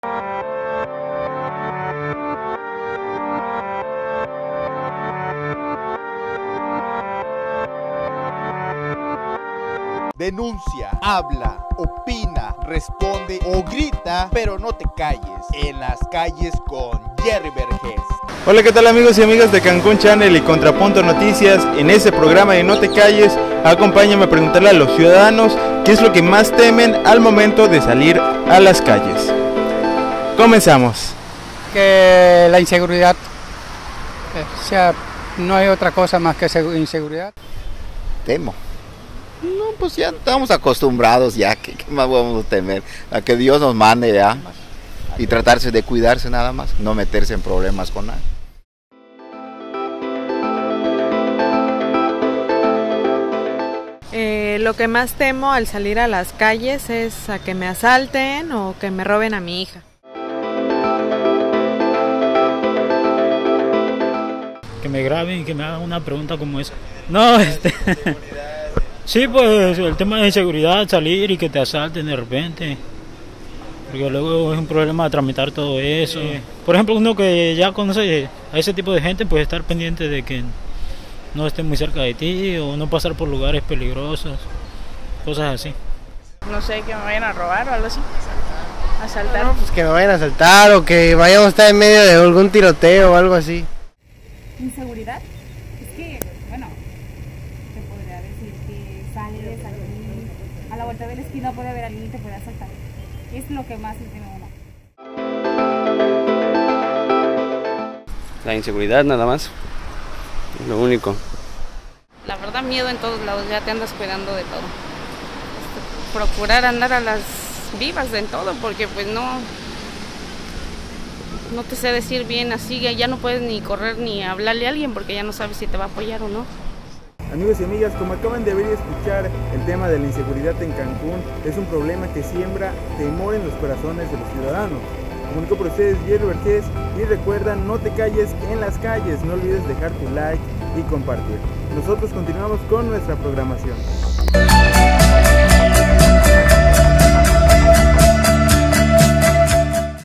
¿QUÉ ES LO QUE DA MIEDO AL SALIR A LAS CALLES?
Muchos cancunenses han modificado sus hábitos a raíz del incremento de inseguridad pero a ti ¿QUÉ ES LO QUE DA MIEDO AL SALIR A LAS CALLES?